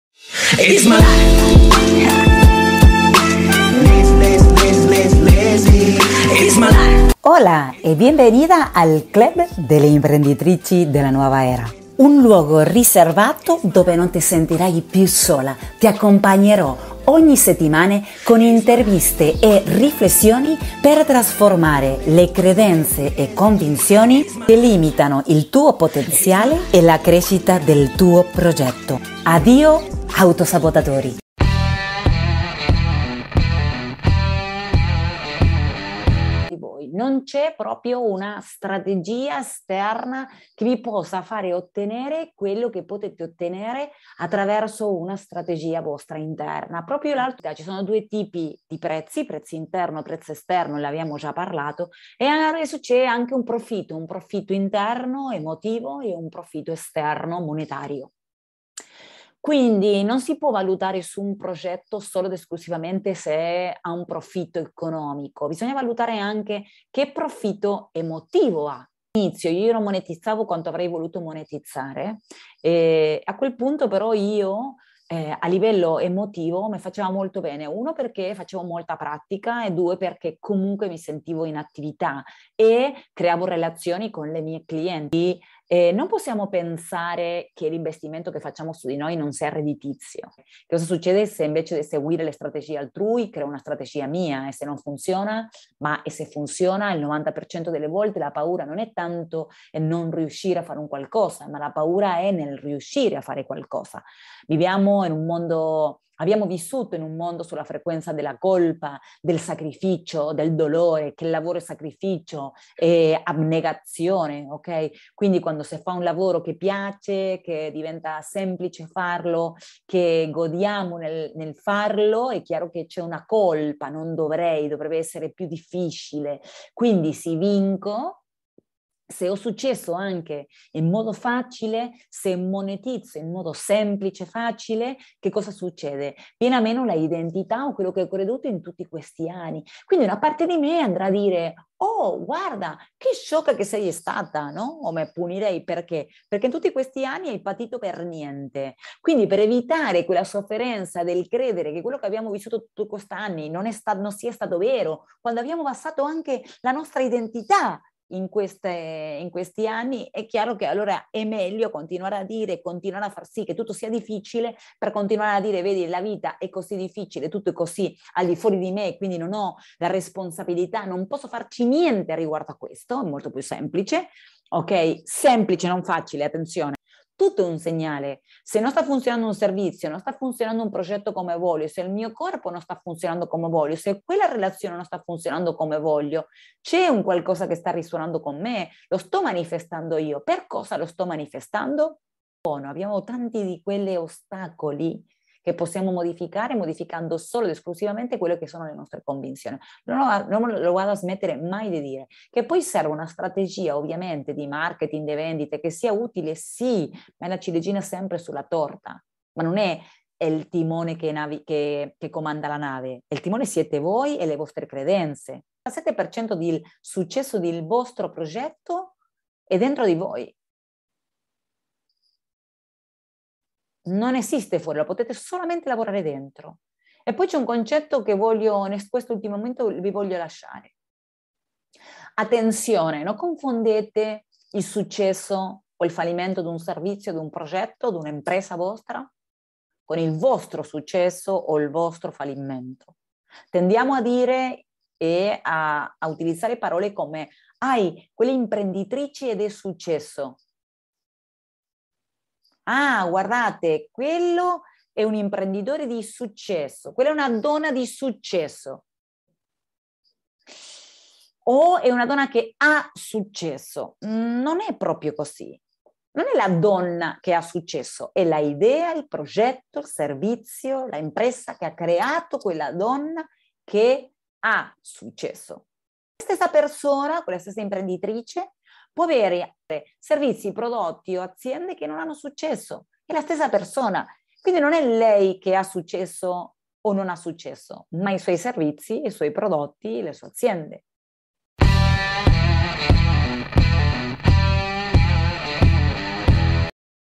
32. Come dare il prezzo giusto ai tuoi servizi
In questo episodio ti spiego quali sono gli errori che solitamente commettono le professioniste che danno servizi di consulenze, coach, terapeuti o consulenti di ogni ambito.